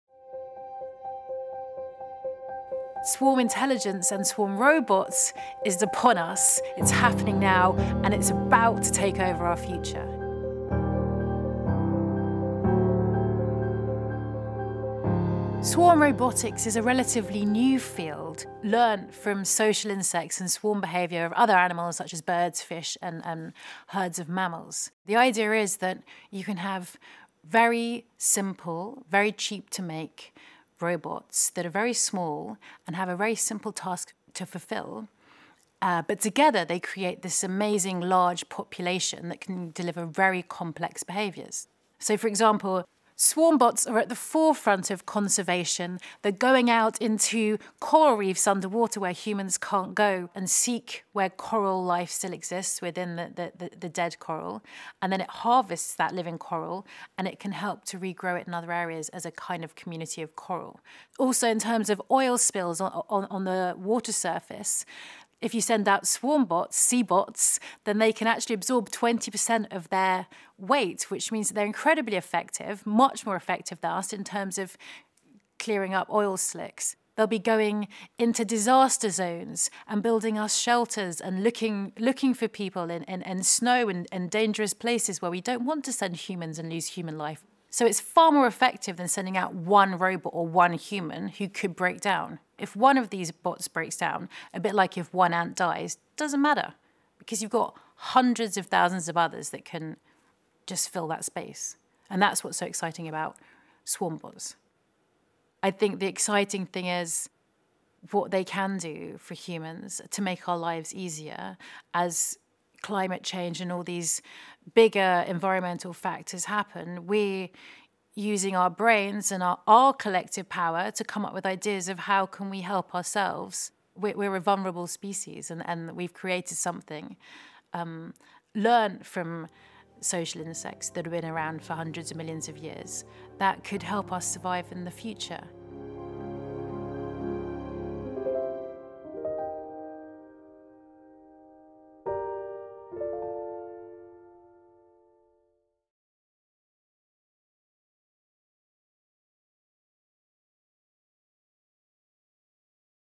Swarmbots: Robots Inspired By Nature | Mega | Bridget Nicholls
Entomologist Bridget Nicholls explains why nature and robotics technology make for powerful allies. Capable of reaching locations out of reach by larger robots and humans swarms of tiny robots, swarmbots, could be deployed to help conserve coral reefs, clean up oil spills, and help in disaster zones. And there are plenty more applications for these little bots.

Like ants or other swarm animals, swarm robots are not impacted by the loss of one unit. Hundreds of thousands of tiny advanced robotic machines working to achieve a defined goal will revolutionise industries and could one day help humanity secure its future.